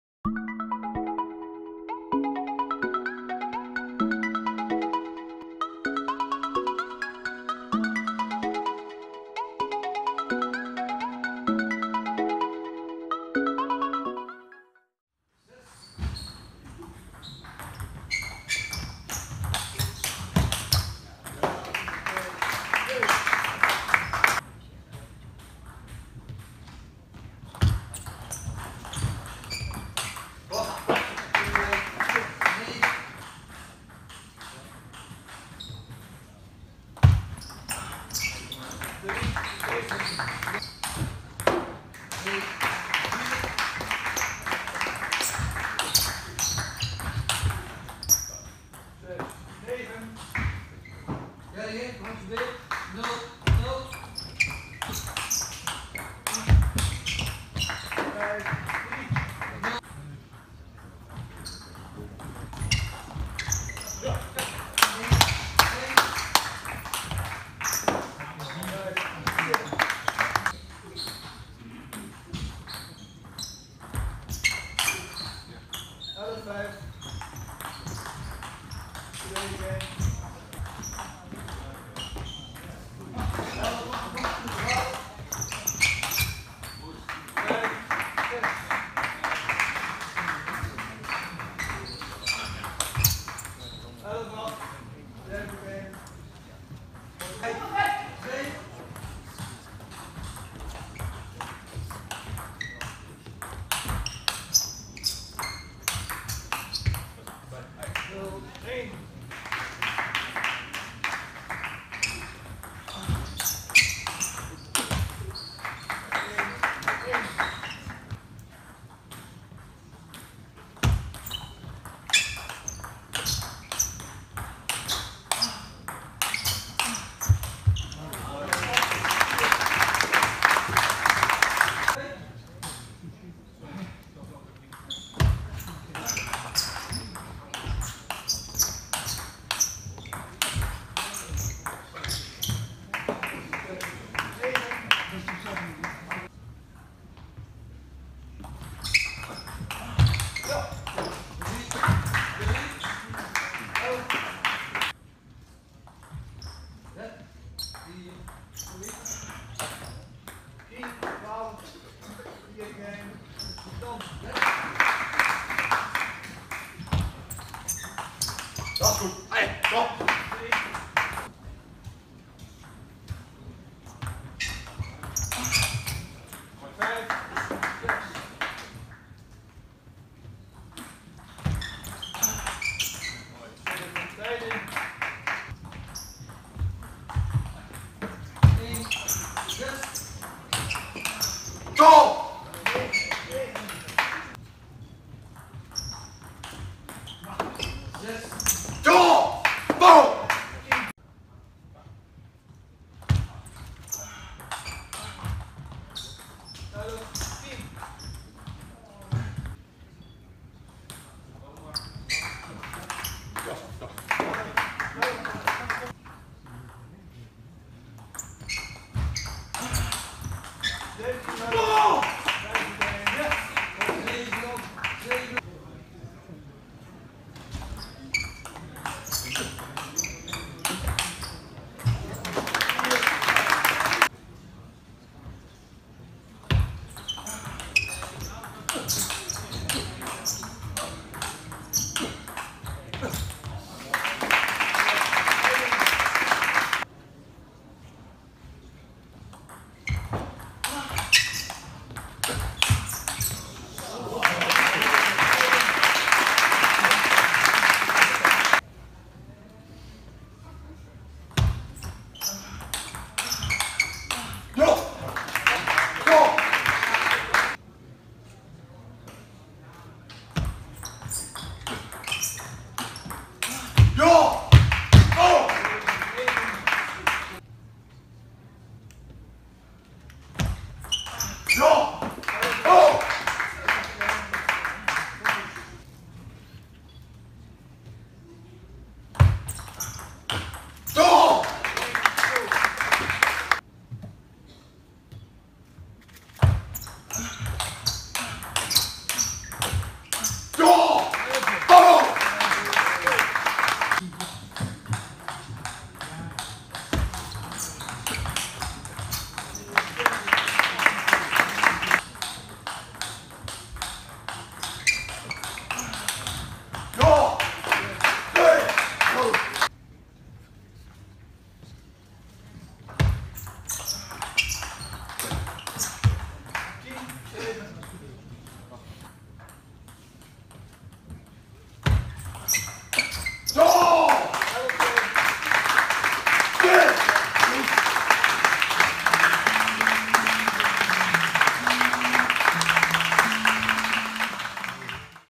Rajko Gommers - Cosmin Stan | Kwartfinale Masters 2017
Privé opnamen. Tafeltennis Masters
zo 22 okt 2017, Leiden, Nederland